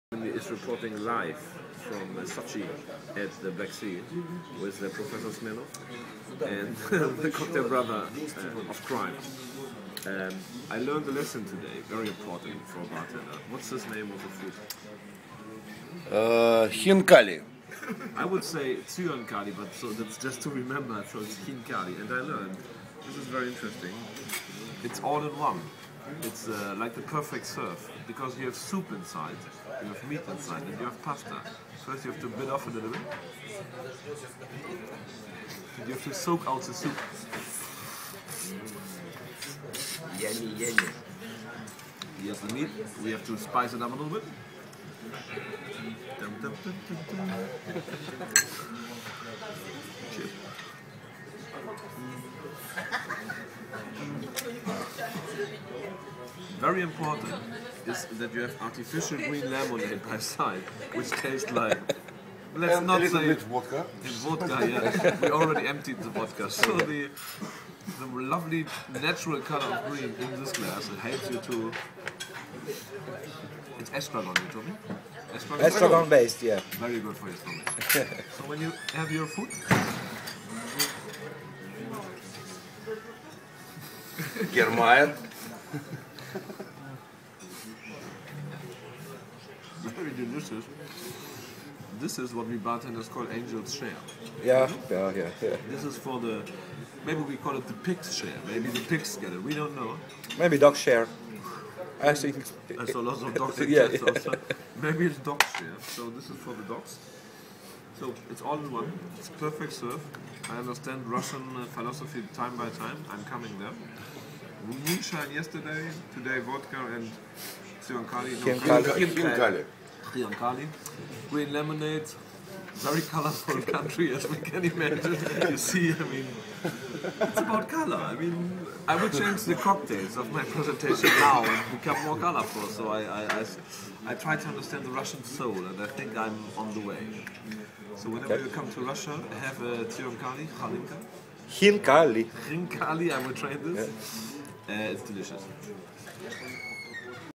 The joy of Russian Lunch - in Sotchi
Professor Smirnoff, Tony and the Cocktail Brohter's of crime explaining me the Russian Lunch "Zyankali..." :) - READ MORE about my Russian ROADTRIP